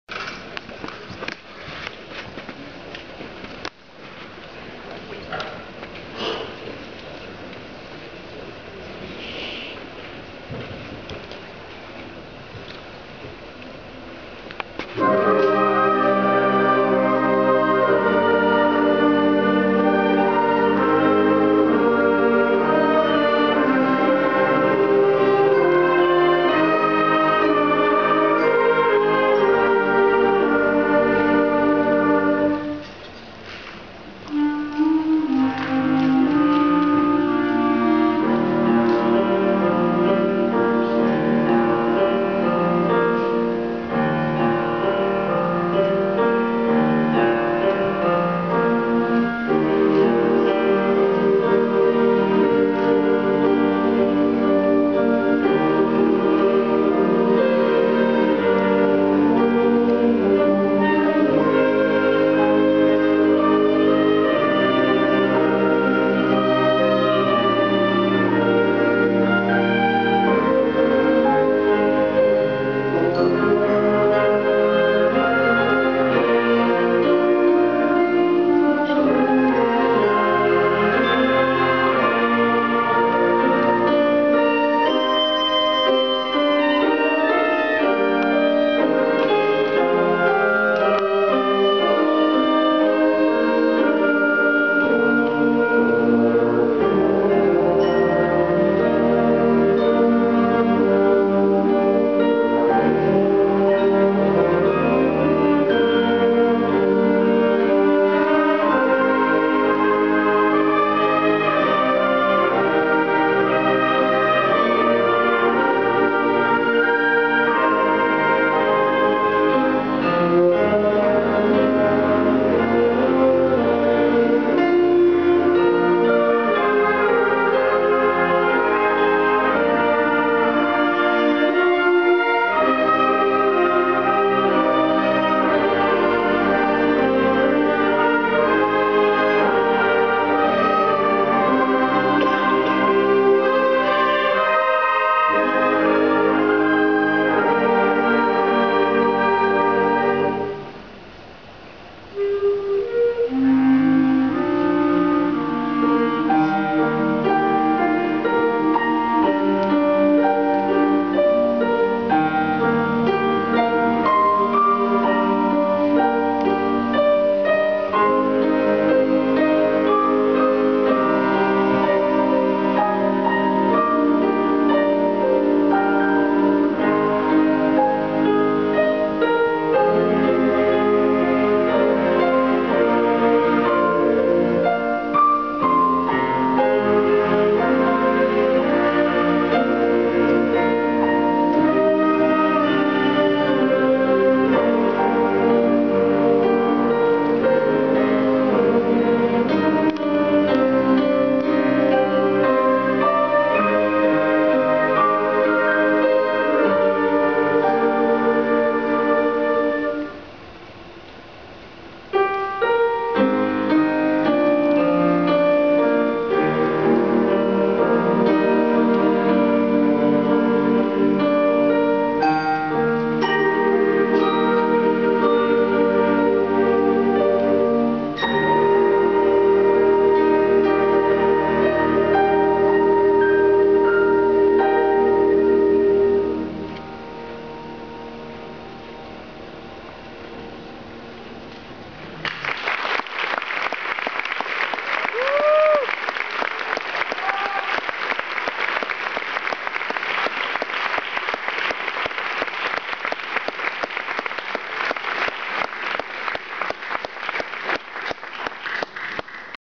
Zach's Piano Solo
Niles West Spring Band Concert.  That's my son on the piano.  (Forgive the sketchy camera work.)